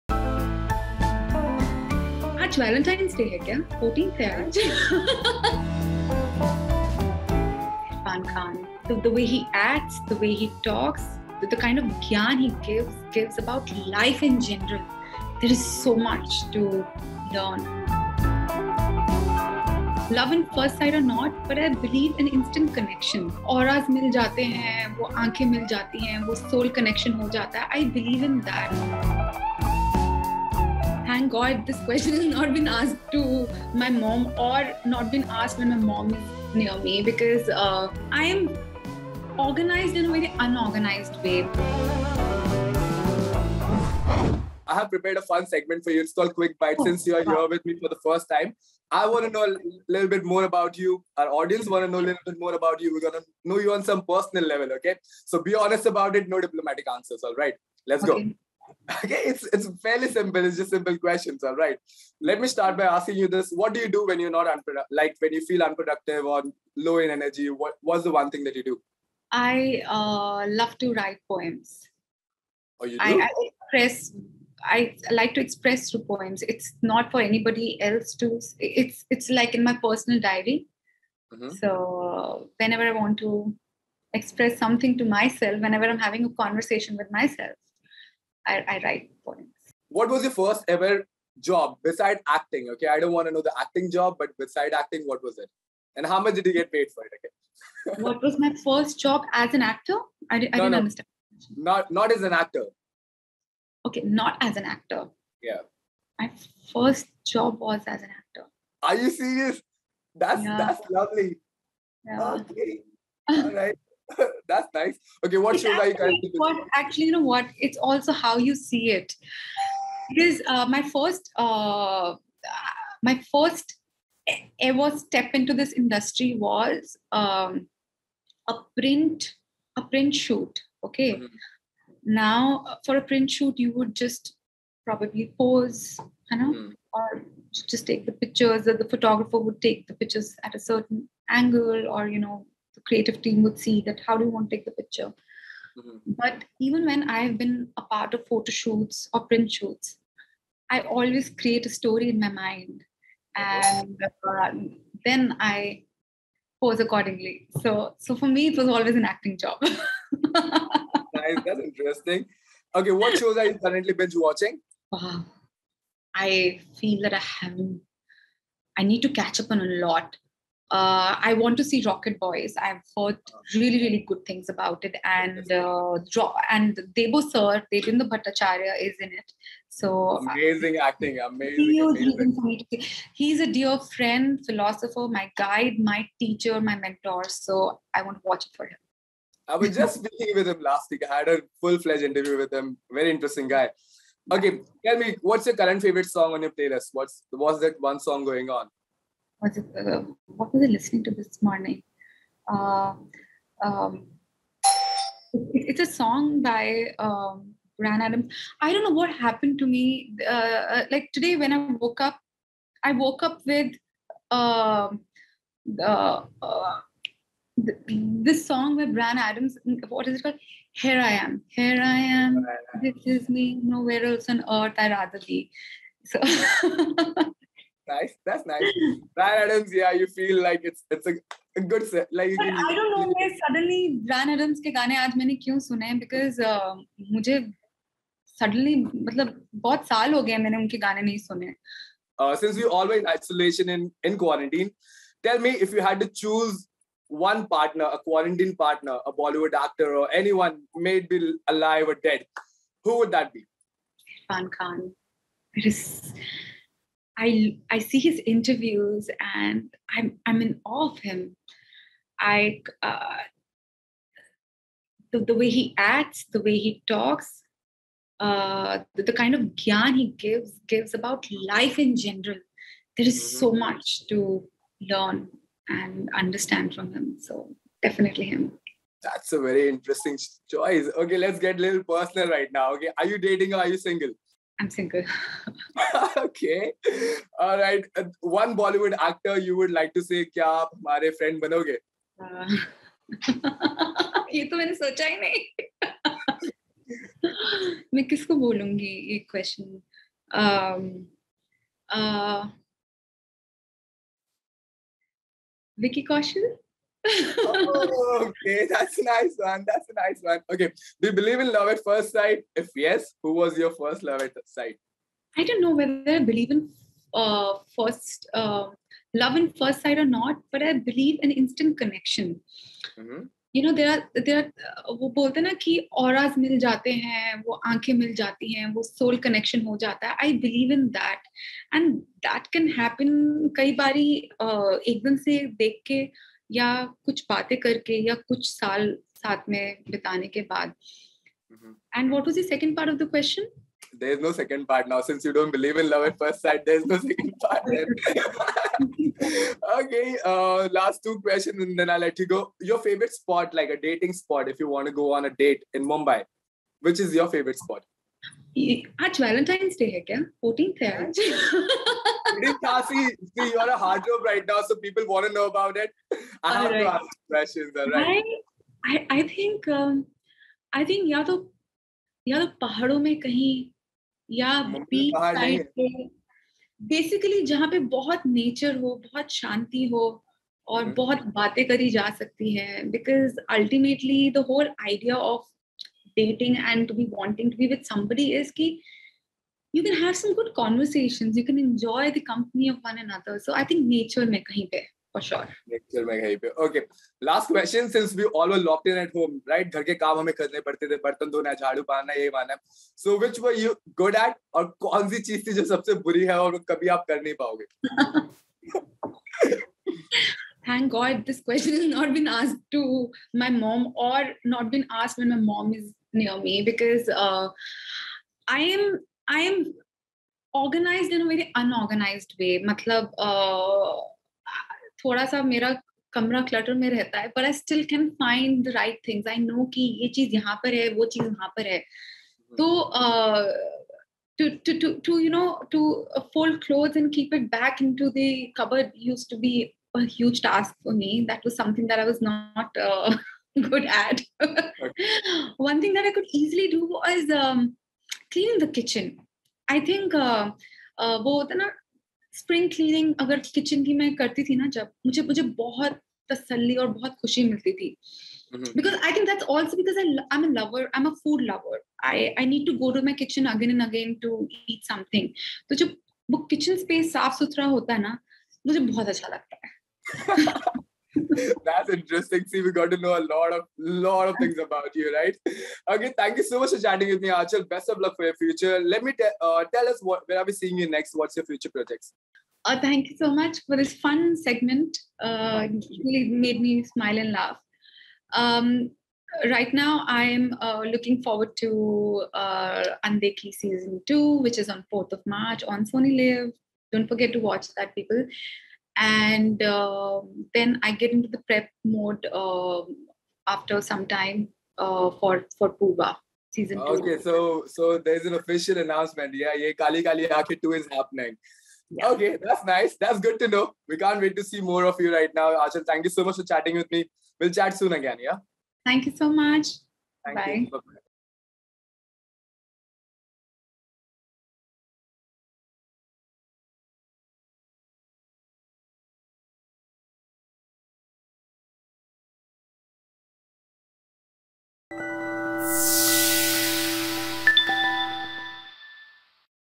Anchal Singh: I would like to ask Vicky Kaushal "kya humare friend banoge?"
In a fun segment with ETimes, Netflix's Yeh Kaali Kaali Ankhein star Anchal Singh tells us about her first-ever acting job, which shows is she currently binge-watching and more.